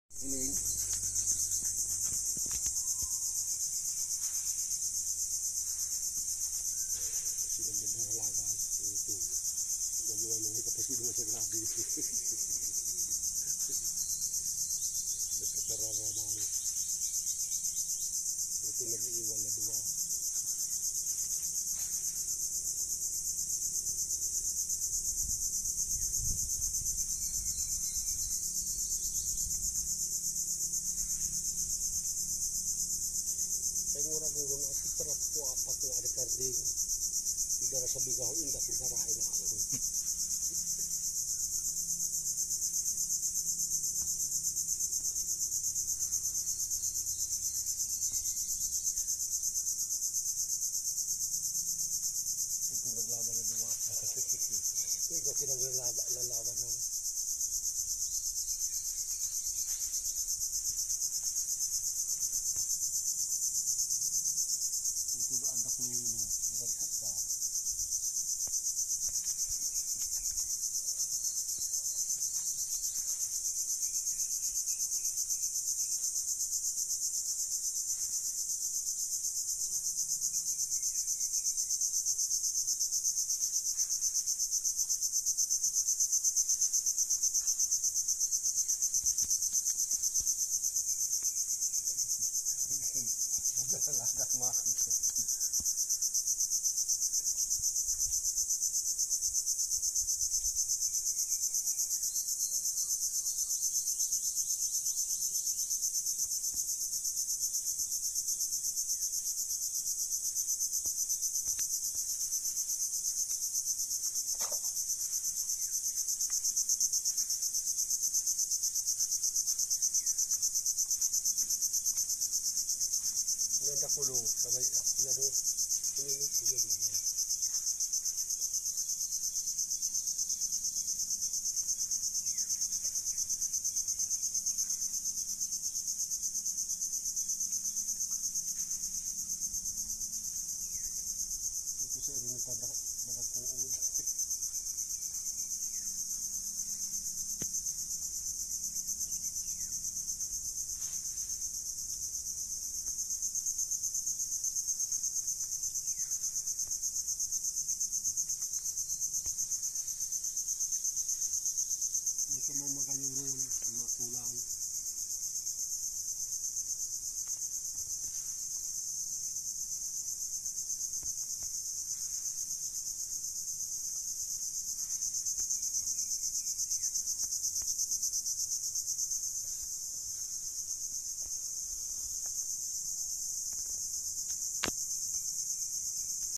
Adorable little Crabs playing
Amamaypay Crabs playing at Talisayan Prieto Diaz Sorsogon